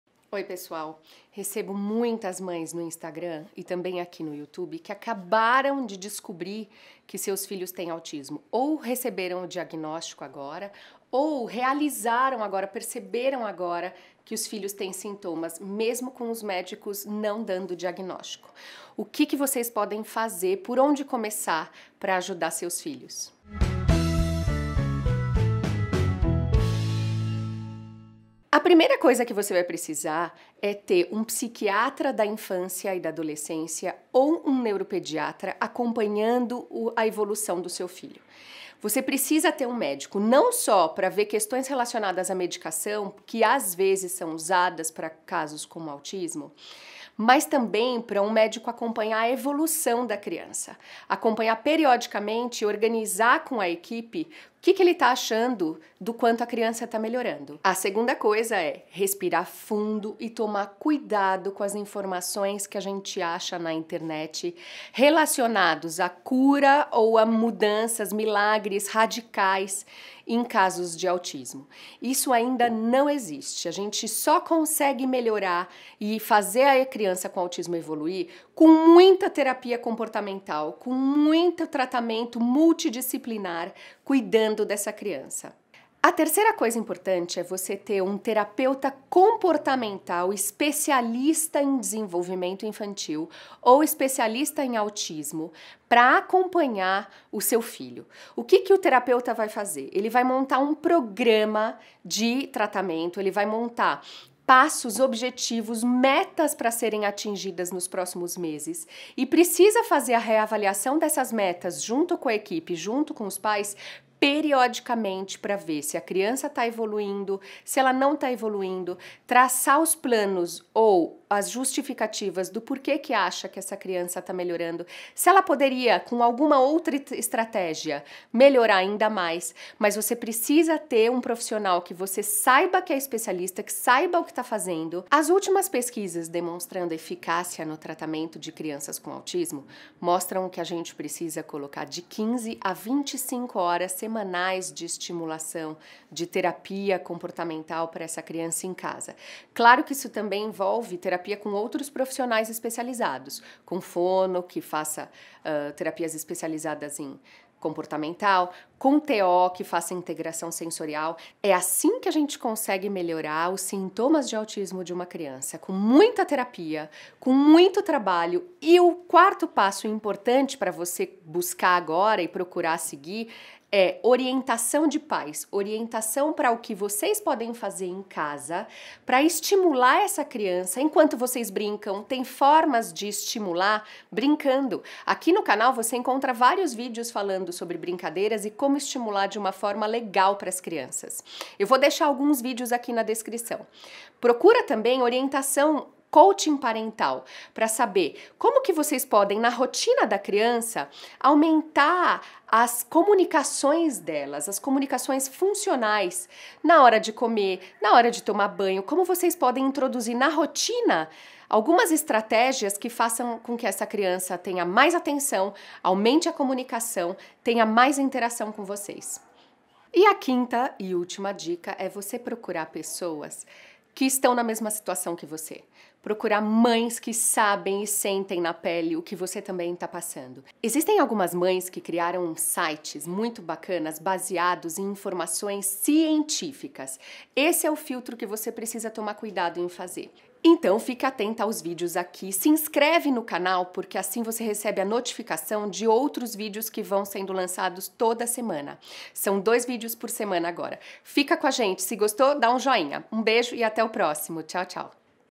Descobrindo o diagnóstico do Autismo - 5 Primeiros Passos
Oi Pessoal!
Muitos pais vem ao consultório desesperados quando descobrem que seus filhos tem autismo. Resolvi fazer este vídeo com os 5 primeiros passos que pais e mães devem tomar após a descoberta do diagnóstico.